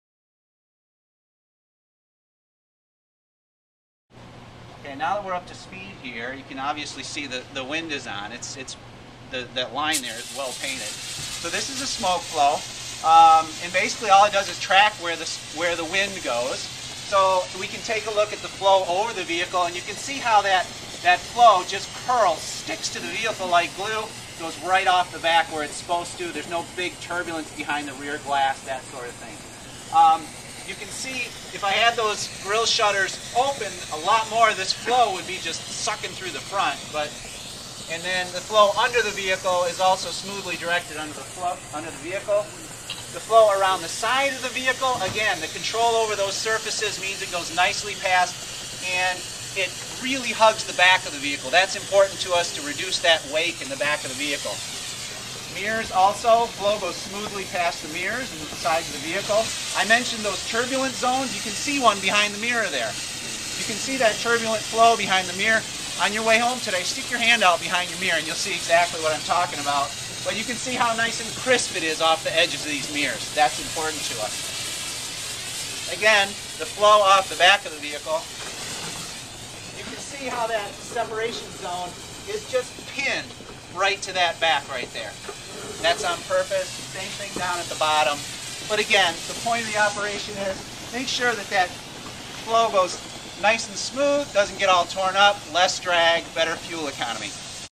Ford Motor Company's Wind Tunnel Aerodynamics Test
As part of the Go Further with Ford conference, they took us to the wind tunnel to show off the aerodynamics of Ford's new cars. This is a simulation using 20-25mph winds and a smoke gun.